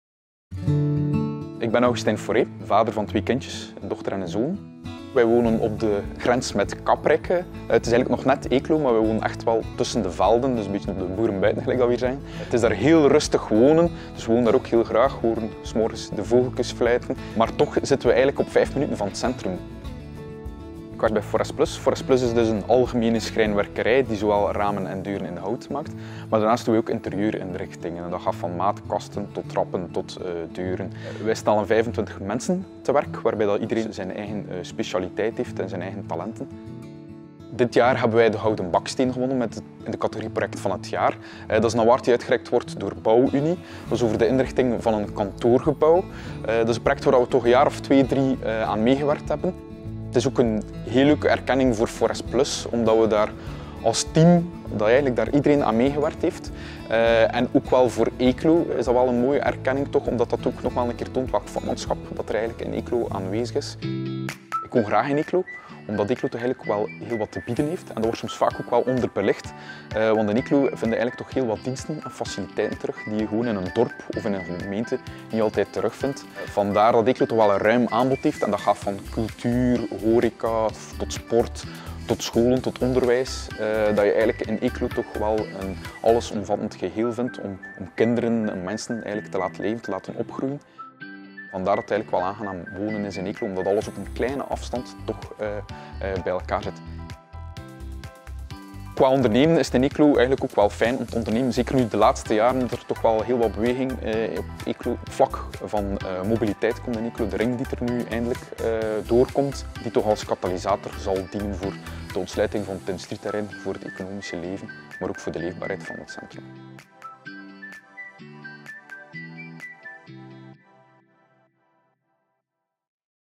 Augustijn Foré Knappe Koppen
Ken je onze Knappe Kop Augustijn al? Ondernemer, vader en trotse inwoner van onze stad.